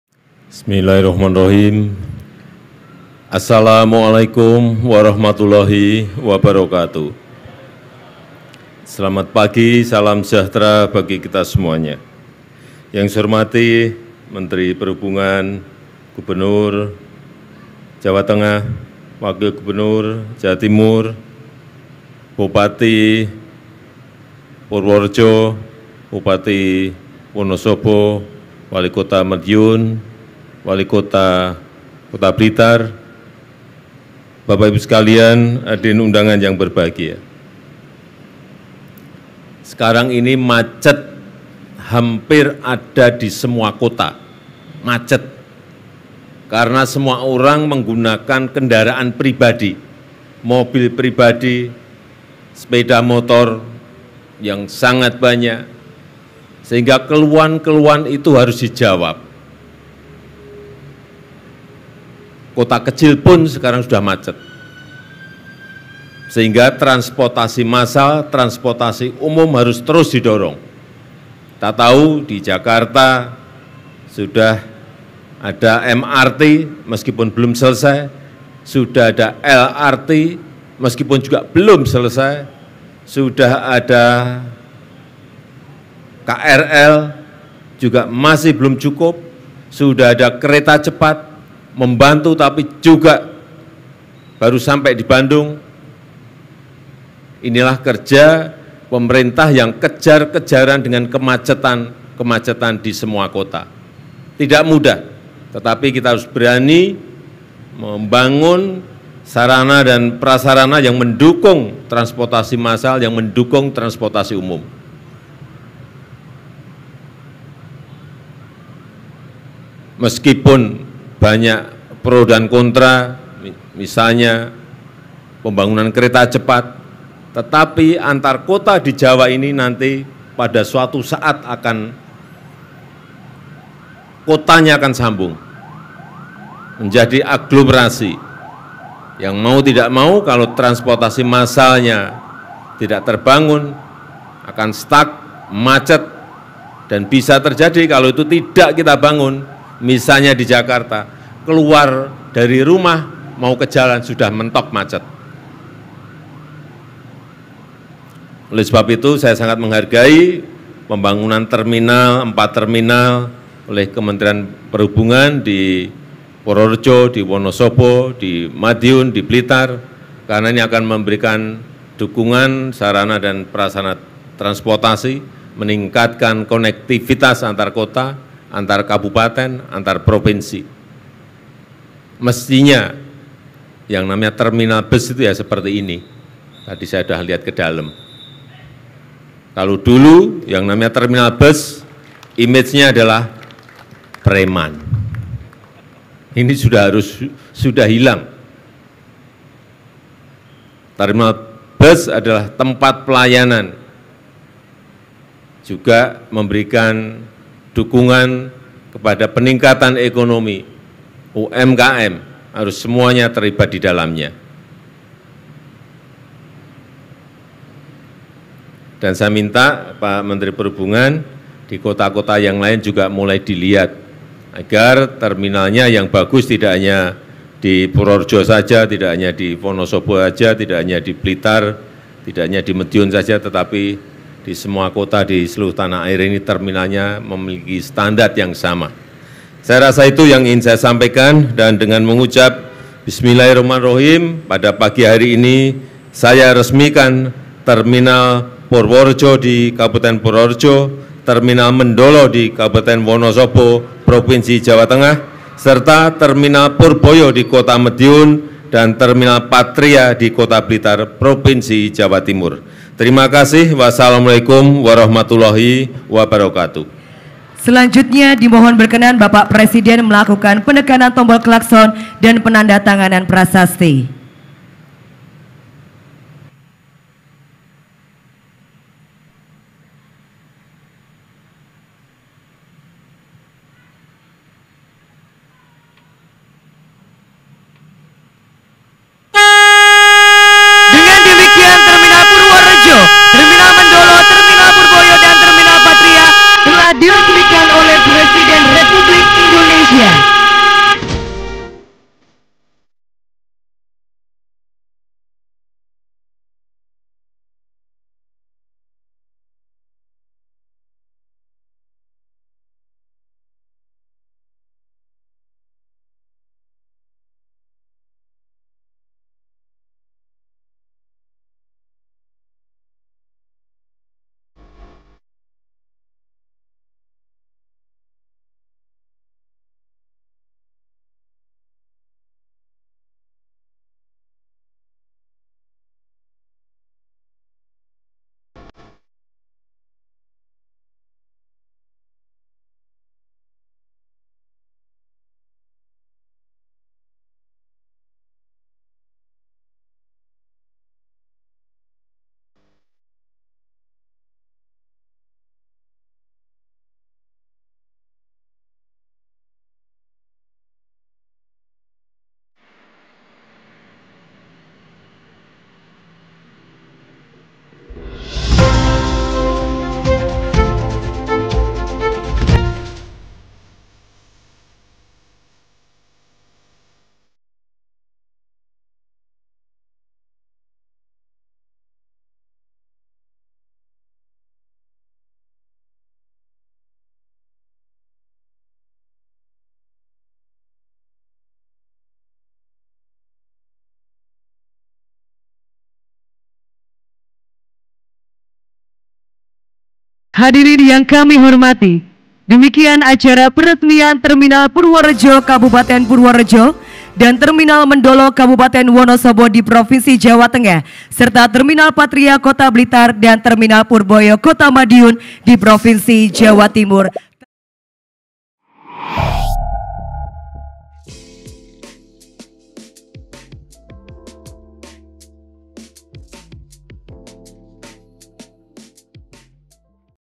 Jokowi Tegas Hapus Citra 'Preman' di Terminal, Reaksi Tawa Iriana Jadi Sorotan
MERDEKA.COM - residen Joko Widodo atau Jokowi meresmikan empat terminal di Jawa Tengah (Jateng) dan Jawa Timur (Jatim), yang dilakukan di Terminal Purworejo, Jawa Tengah, Selasa (2/1). Dalam peresmian, Jokowi didampingi ibu negara Iriana Jokowi.

Selain di Purworejo, tiga terminal yang diresmikan Jokowi secara hybrid antara lain, Terminal Mendolo di Kabupaten Wonosobo, Terminal Purboyo di Kota Madiun, dan Terminal Patria di Kota Blitar.

Jokowi mengapresiasi pembangunan empat terminal tersebut karena bisa mengubah wajah terminal bus yang dulunya identik dengan premanisme. Menurut dia, terminal bus harus menjadi tempat pelayanan bagi pengguna transportasi umum.